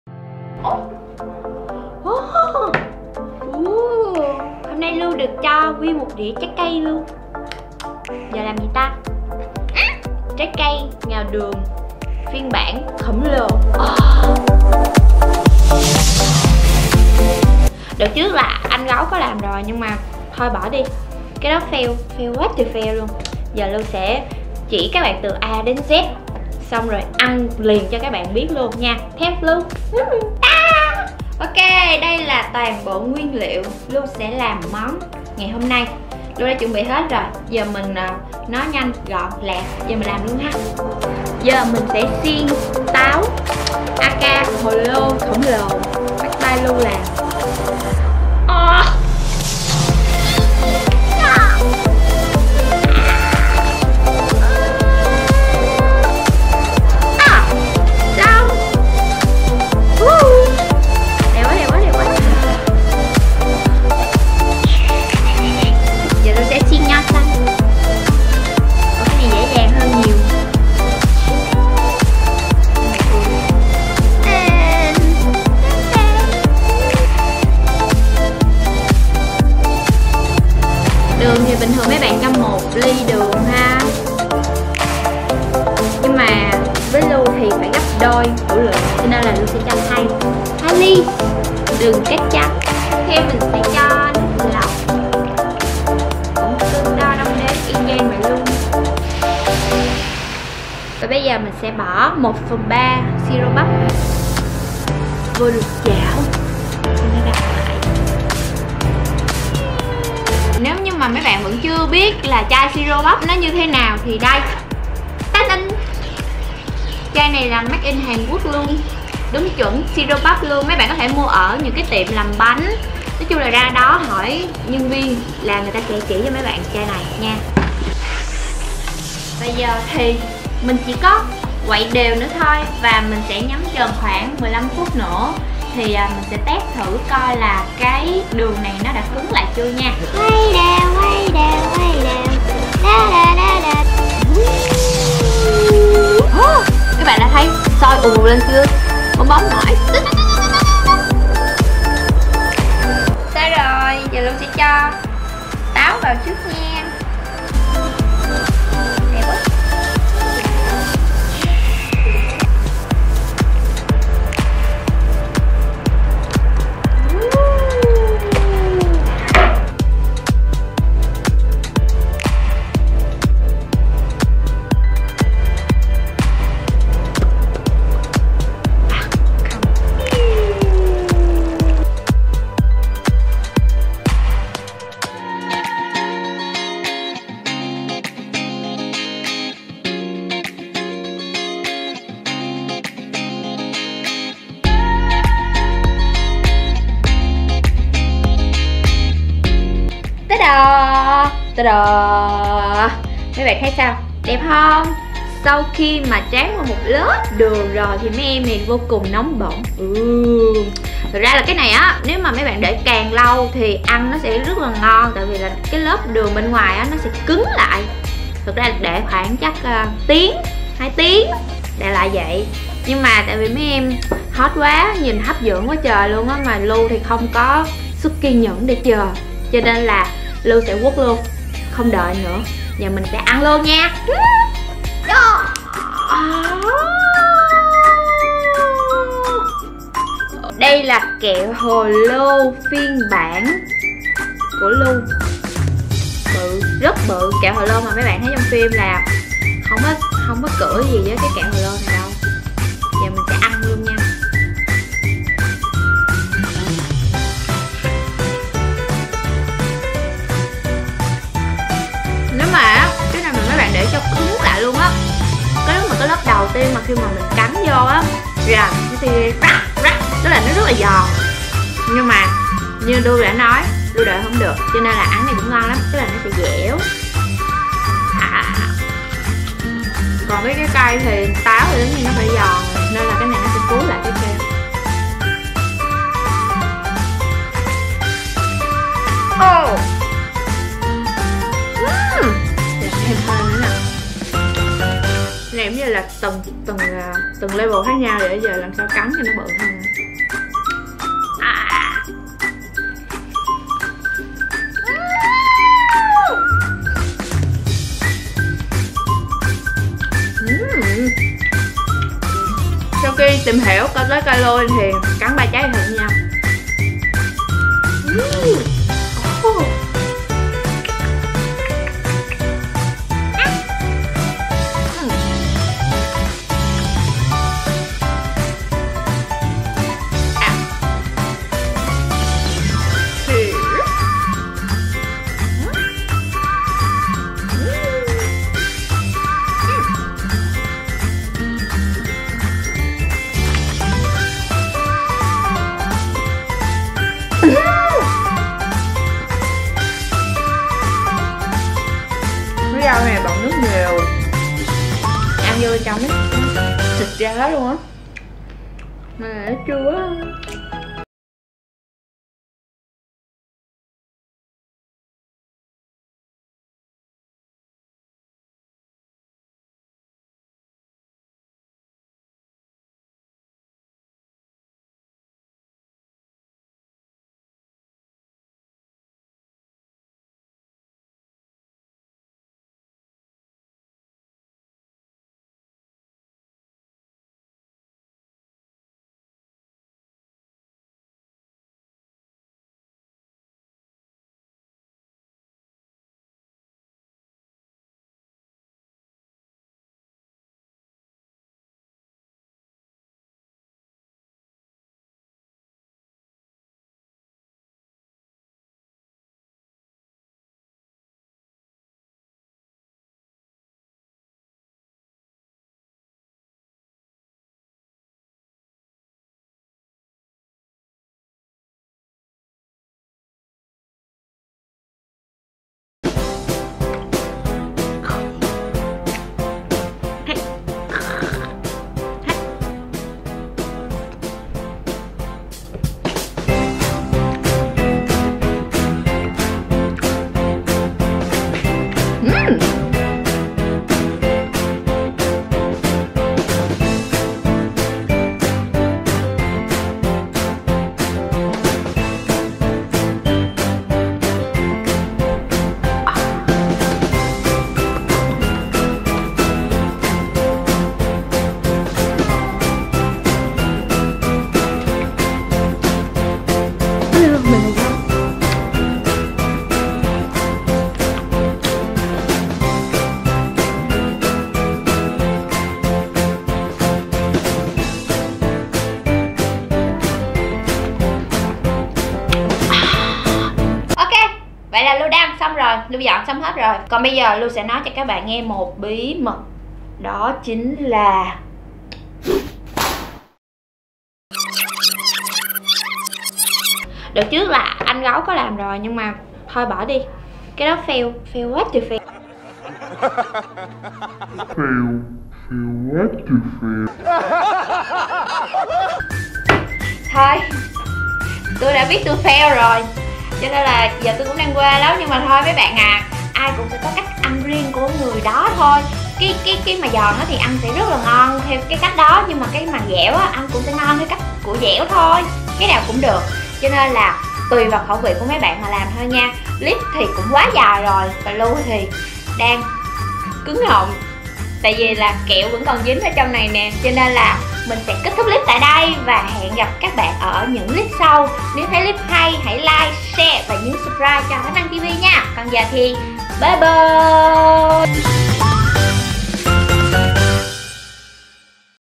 HOW TO MAKE CANDIED FRUITS TANGHULU | LU LÀM KẸO HỒ LÔ NGÀO ĐƯỜNG VÀ CÁI KẾT BẤT NGỜ | THÁNH ĂN TV
HOW TO MAKE CANDIED FRUITS *TANGHULU* | LU LÀM KẸO HỒ LÔ NGÀO ĐƯỜNG VÀ KẾT QUẢ BẤT NGỜ | THÁNH ĂN TV

Hôm nay Lu trổ tài làm kẹo hồ lô trái cây, cùng xem Lu có thành công không nha!

🌏GIAO LƯU VỚI LU TẠI HÀNH TINH HAM ĂN (Group Facebook)
© Bản quyền thuộc về THÁNH ĂN TV
© Copyright by THÁNH ĂN TV ☞ Do not Reup